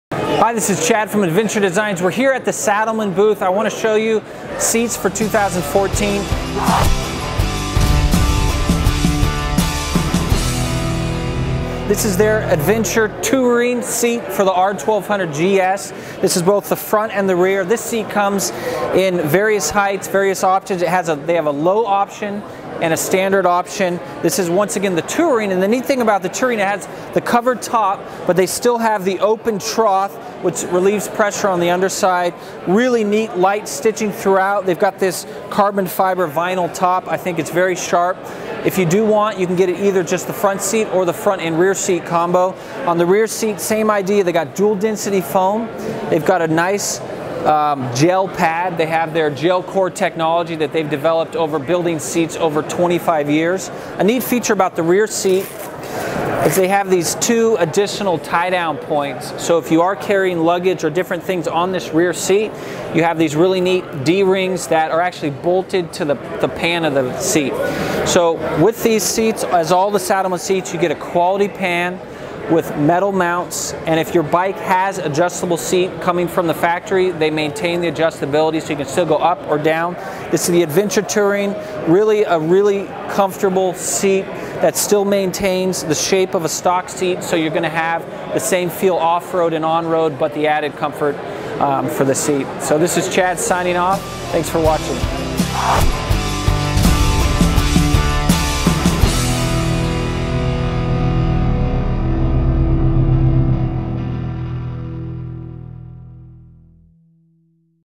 Saddlemen Adventure Tour Seats for 2014 by Adventure Designs
Chad Olson of explains the features and benefits of the new Saddlemen Adventure Tour seats which have been available for just over a year now.

Cheers,
Chad & the Adventure Designs Team